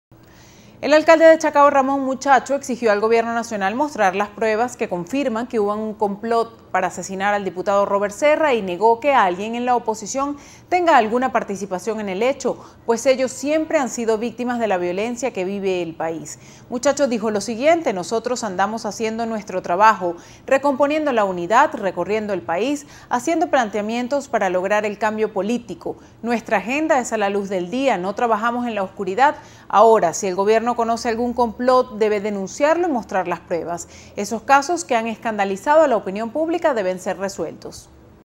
Ramón Muchacho exige pruebas del Caso Serra
El alcalde de Chacao exigió al gobierno nacional demostrar que hubo un complot para asesinar al diputado oficialista.